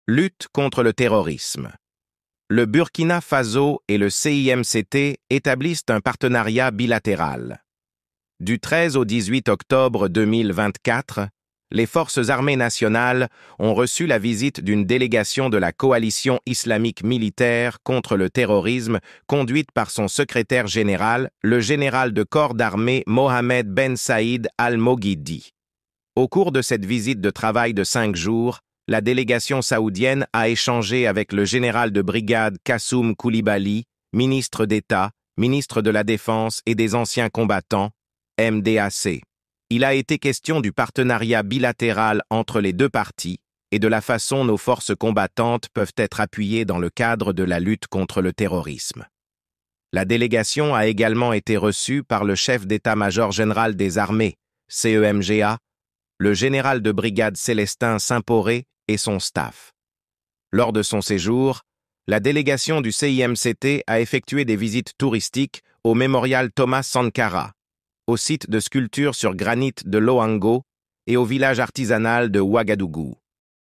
Lutte contre le terrorisme : le Burkina Faso et le CIMCT établissent un partenariat bilatéral
Du 13 au 18 octobre 2024, les Forces Armées Nationales ont reçu la visite d'une délégation de la Coalition Islamique Militaire Contre le Terrorisme, conduite par son Secrétaire Général, le Général de Corps d'Armée Mohammed Ben Saïd Al Moghidi.
Au cours de cette visite de travail de cinq jours, la délégation saoudienne a échangé avec le Général de Brigade  Kassoum Coulibaly, ministre d'Etat, ministre de la Défense et des Anciens Combattants (MDAC). Il a été question du partenariat bilatéral entre les deux parties et de la façon nos forces combattantes peuvent être appuyées dans le cadre de la lutte contre le terrorisme.
La délégation a également été reçue par le Chef d’Etat-Major General des Armées (CEMGA), le Général de Brigade Célestin Simporé et son staff. Lors de son séjour, la délégation du CIMCT a effectué des visites touristiques au mémorial Thomas Sankara, au site de sculptures sur granite de Loango et au village artisanal de Ouagadougou.